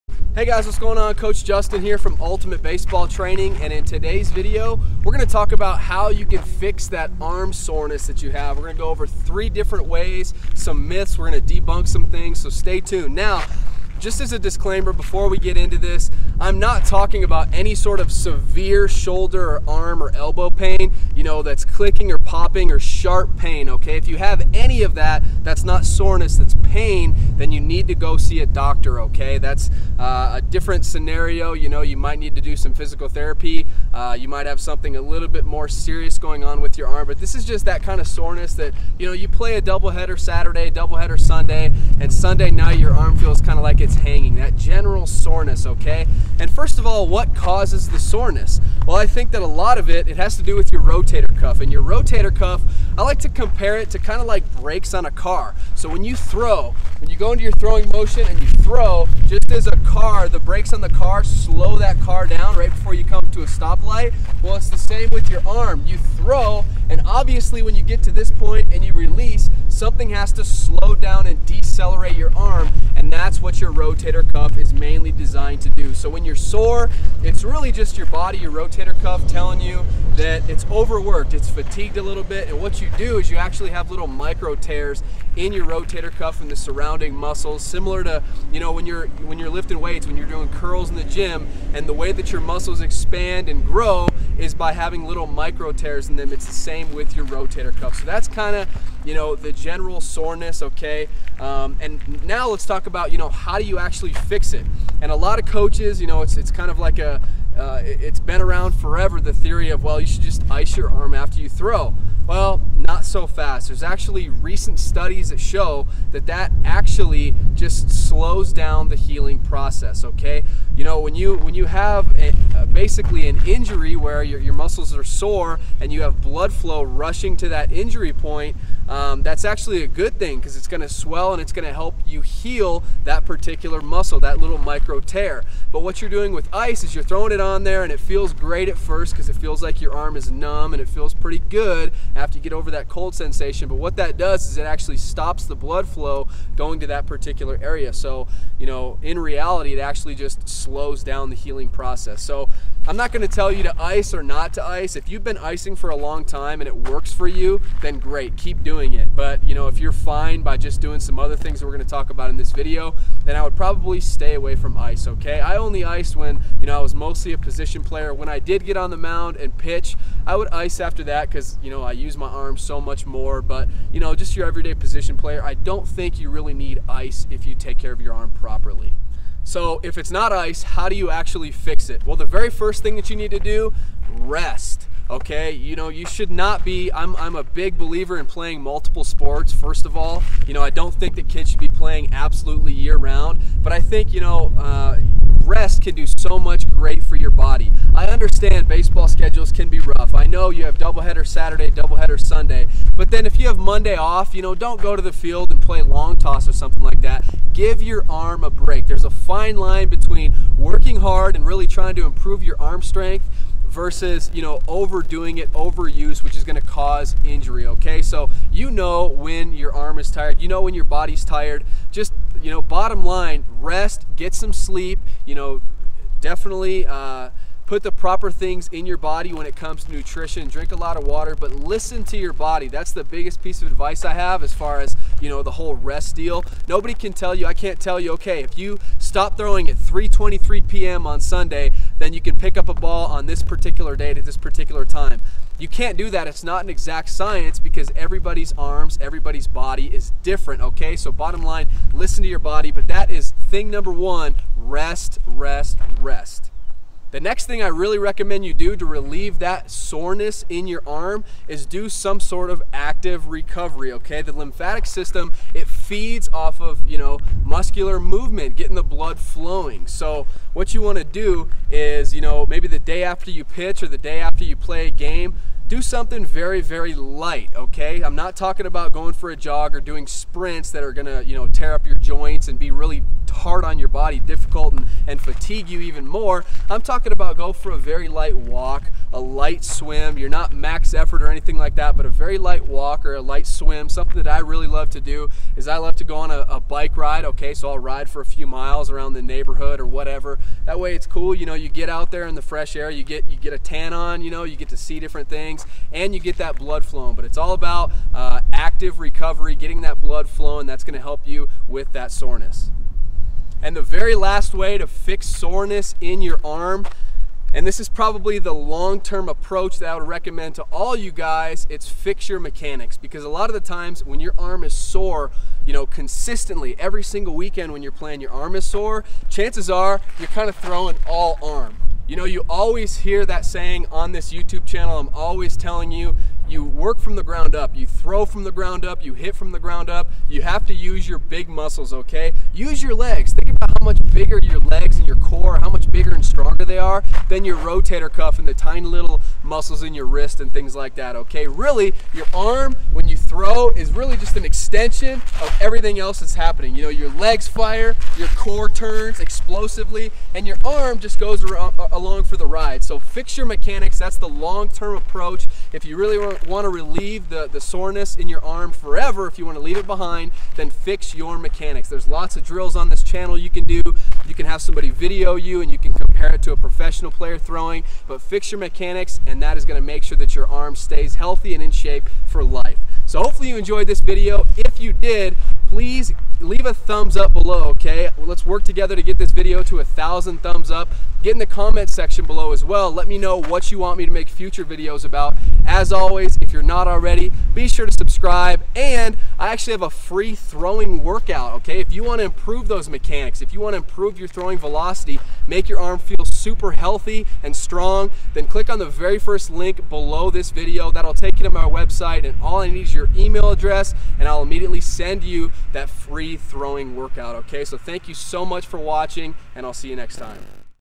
How To FIX ARM SORENESS In Baseball (3 WAYS!)
How To FIX ARM SORENESS In Baseball (3 WAYS!) In today's video, Coach Justin is going to show you how to fix arm soreness so that you can stay healthy and playing at your best!